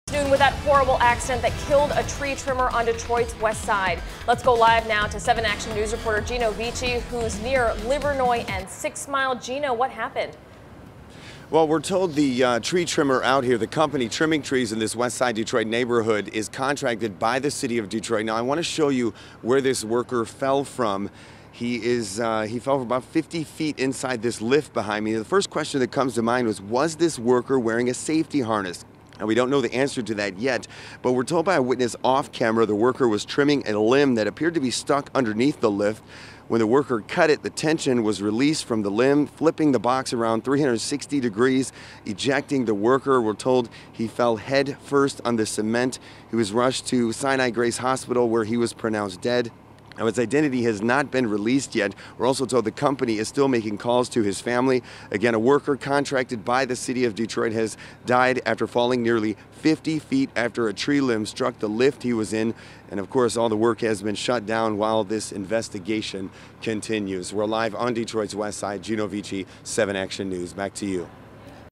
Tree trimmer dies after fall while cutting trees at Detroit home
Detroit police say a man trimming trees at a home in Detroit died when he fell from the bucket of his truck.  The worker for All Metro Tree Service was flipped out of his box when he cut the limb.  It happened around 10:30 a.m. near Margareta between Santa Rose and Stoepel on the city's west side. That's near 7 Mile and Livernois.  Right now, the worker has not been identified as they wait to notify his next of kin.